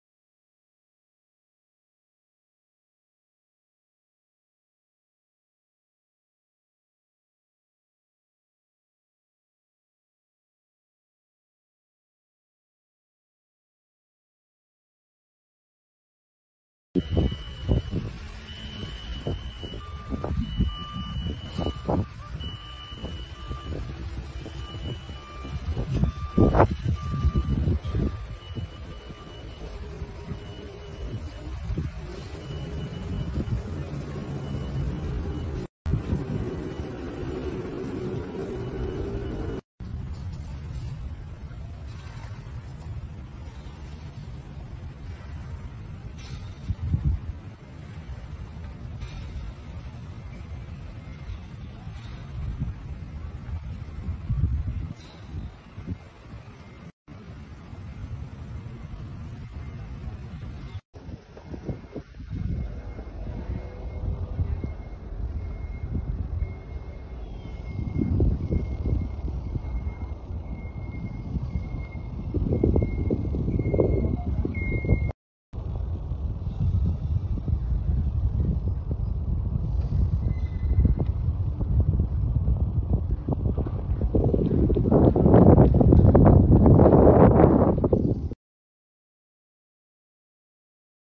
BERKELASSS SIRKUIT FORMULA E JAKARTA || BANNER SPONSOR & PADDOCK TERPASANG RAPIH
UPDATE PENGERJAAN SIRKUIT FORMULA E JAKARTA